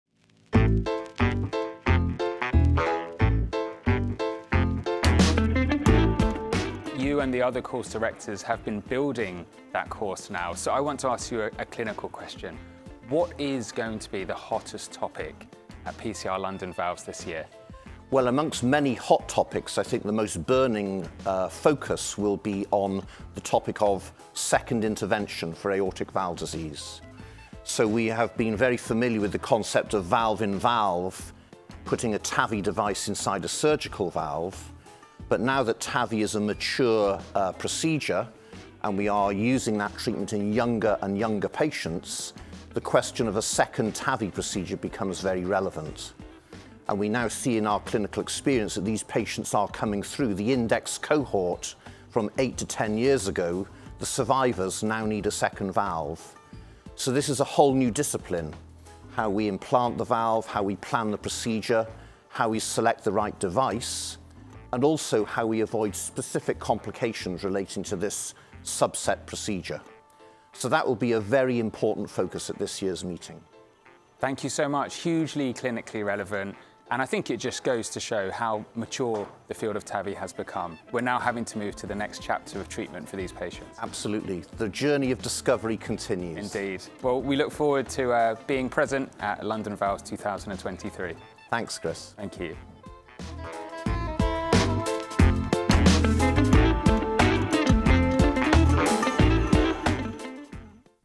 Beyond TAVI: The future takes shape at PCR London Valves 2023
The field of TAVI is evolving, and PCR London Valves 2023 is at the forefront, bringing you the latest insights and innovations. From valve-in-valve procedures to the emerging need for second TAVI interventions, discover how this year's conference is pushing the boundaries of cardiovascular medicine in this short interview with Course Director Bernard Prendergast.